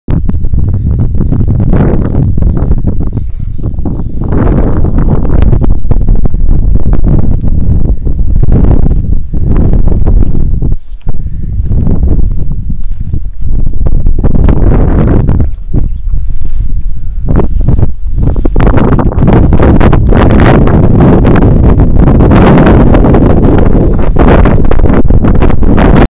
Villa Epecuen, Ciudad Fantasma
una ciudad floreciente que fue tapada por el agua salada y sus niveles de altura bajaron y ahora se puede apreciar el dano irreparable.